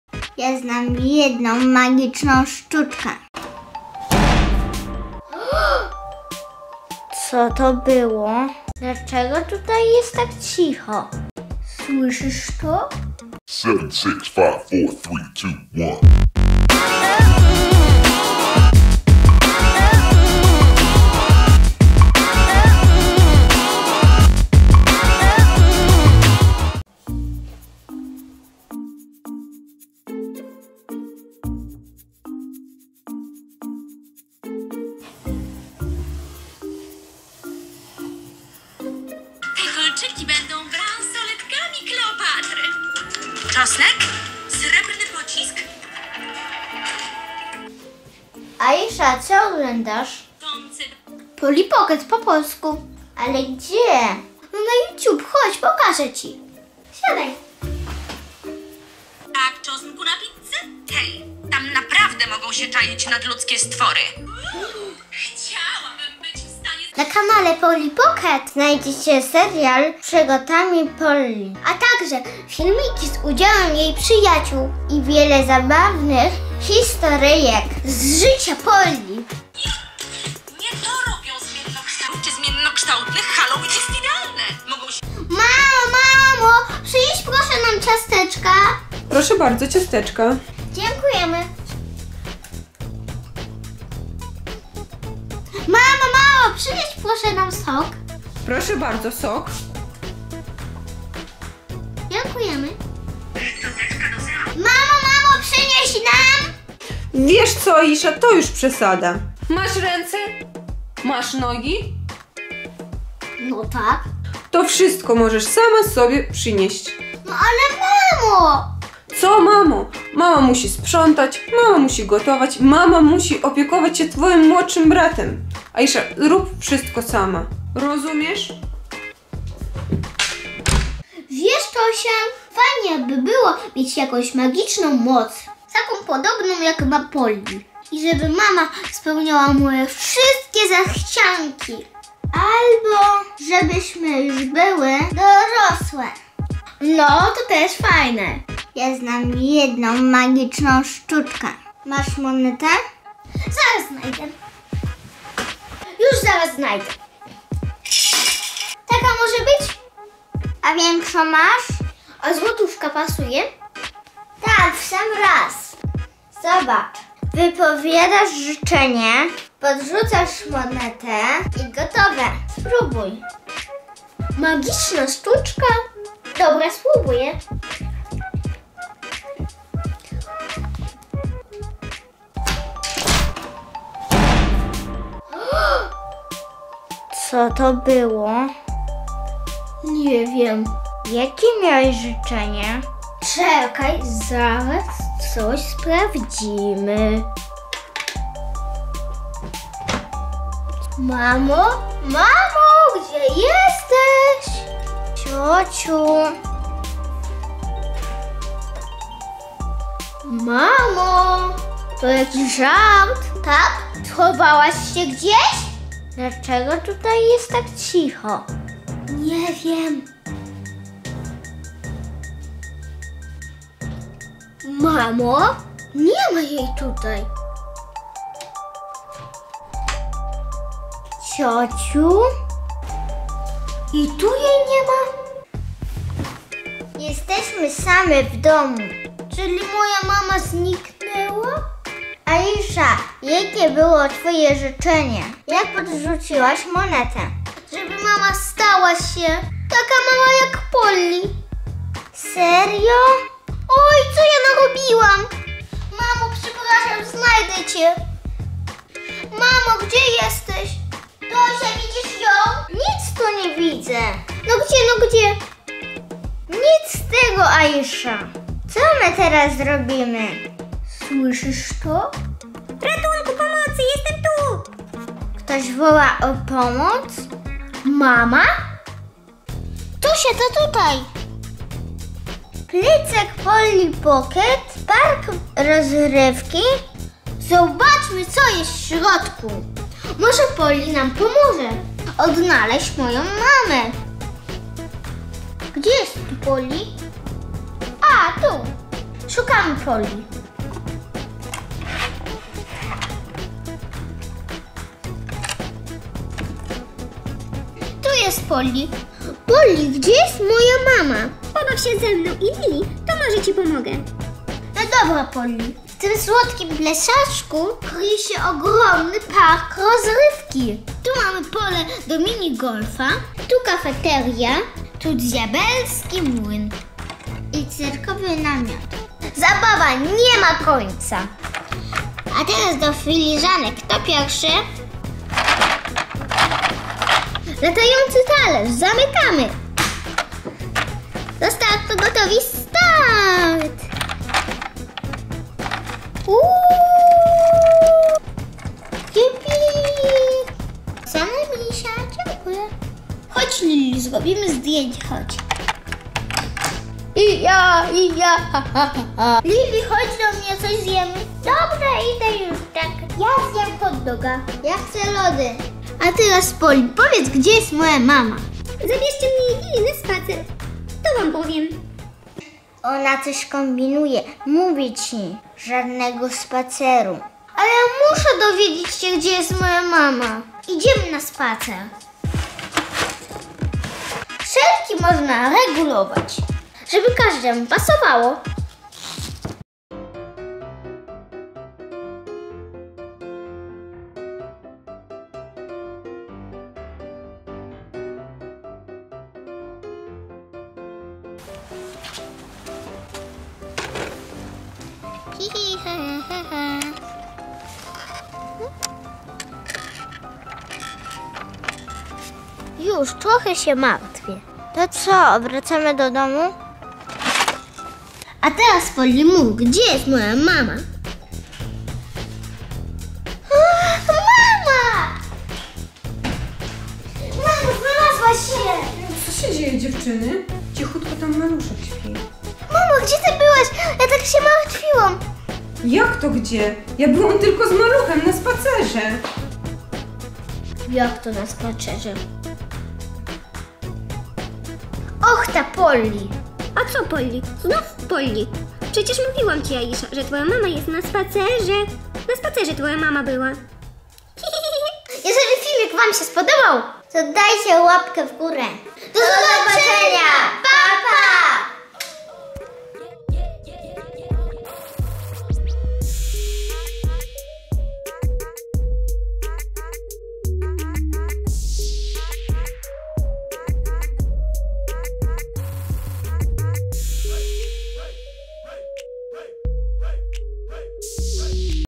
Mama Zniknęła - Czy Polly Pocket nam Pomoże?!
SAME W DOMU! Czy MAMA Zniknęła?!
Zostałyśmy same w domu - Czy ktoś nas śledzi? Gdzie jest mama?